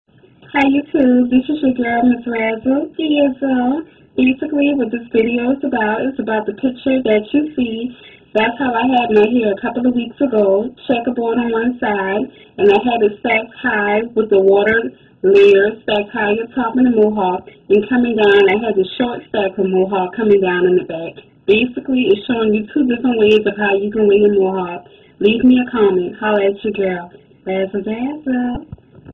M$. Razzel Dazzle Present's The Checker Board Mo Hawk Picture
M$. Dazzle Talking About The Checker Board Mo Hawk Picture Picture That I Recently Had. In The Photo You see a Checker Board Style, on One Side with The High Water Fall's Stacked High In Layer's up Top. Along with The Short Stacked Mo Hawk Comming in The Back. On The Other Side I Have a Heart.